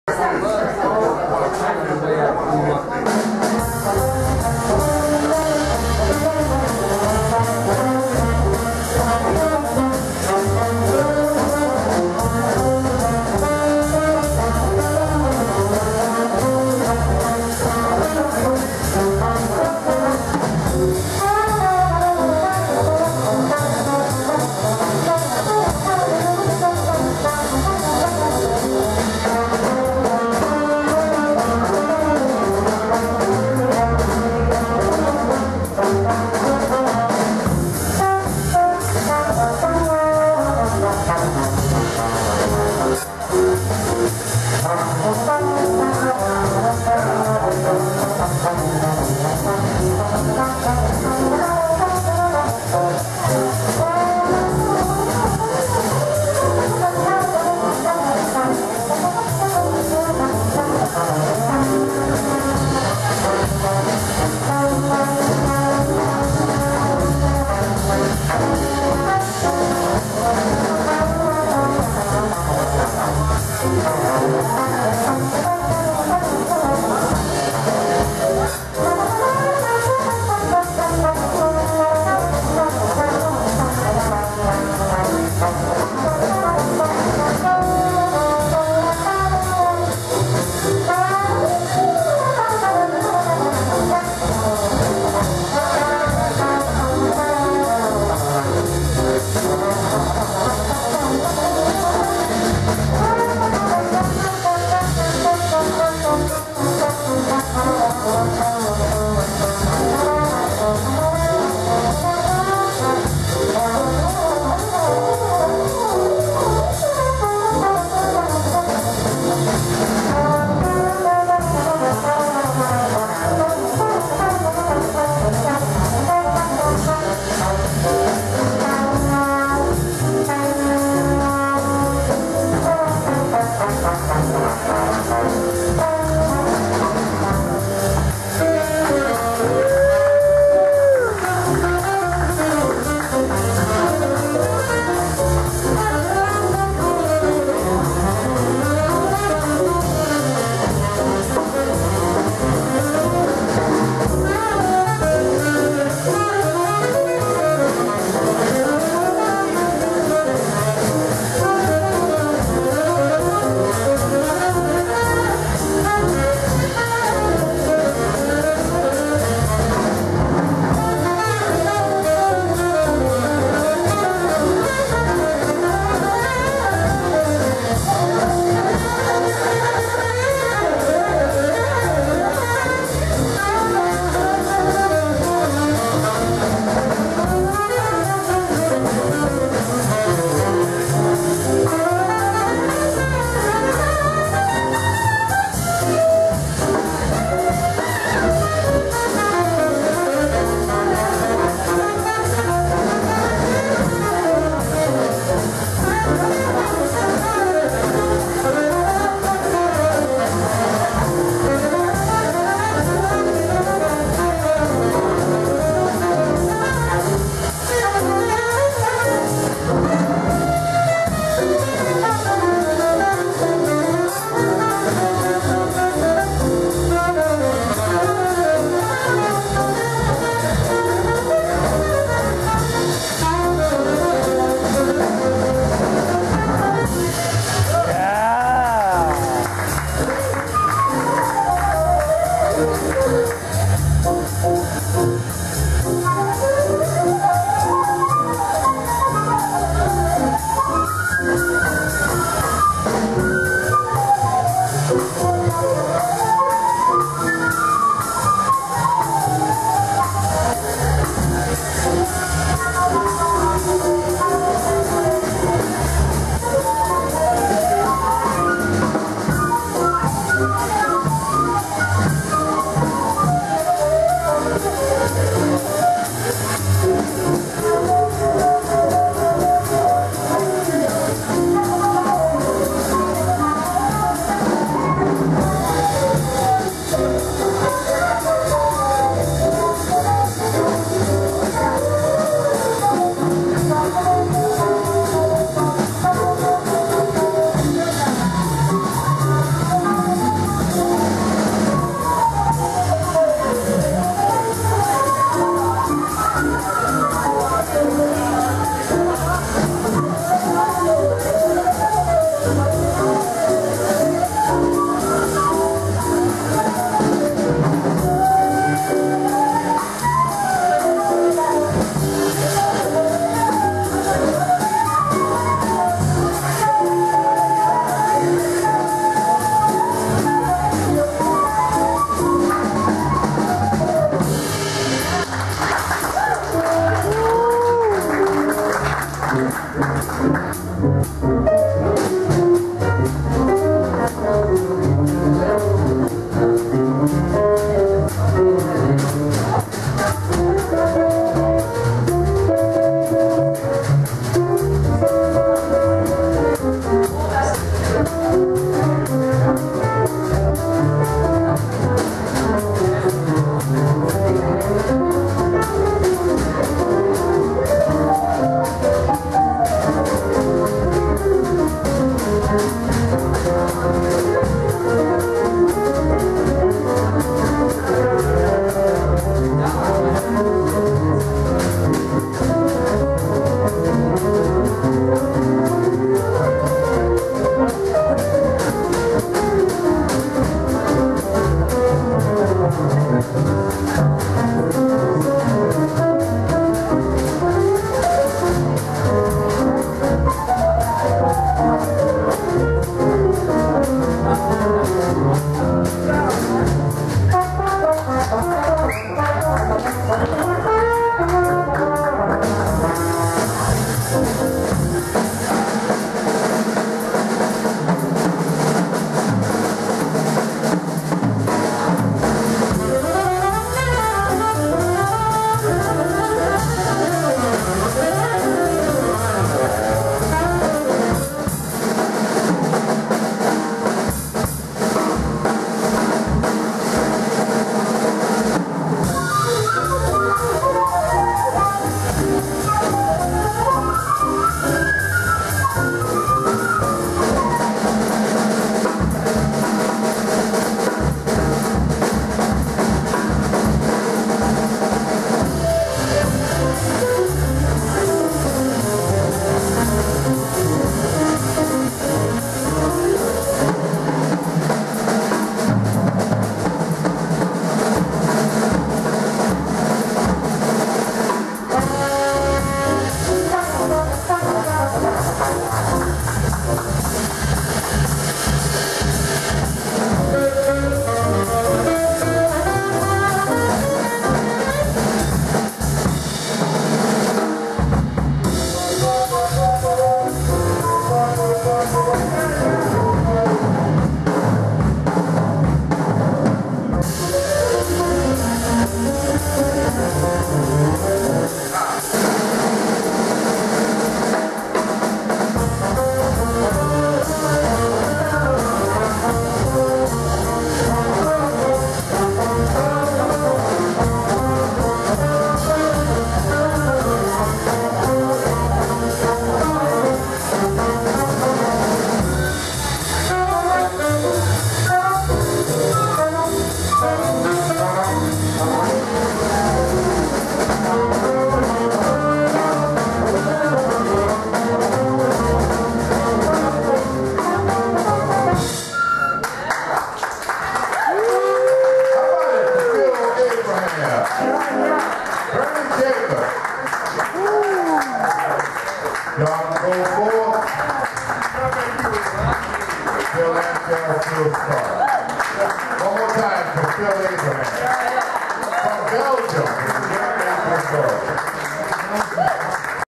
Trombone virtuoso, Phil Abraham, with Clarence Acox 8B / 8
May 2013
Clarence Acox's Legacy Quartet celebrated its 28th anniversary of playing at the New Orleans Creole by laying down a groove, with Phil, a few hours before Phil's flight back to Europe. Among the luminaries of the evening:
Clarence Acox (d)
John Goforth (tenor)
Bernie Jacobs (flute, vocals, alto)
Phil Sparks (db)
Bill Anschell (p)
Stephanie Porter (vocals)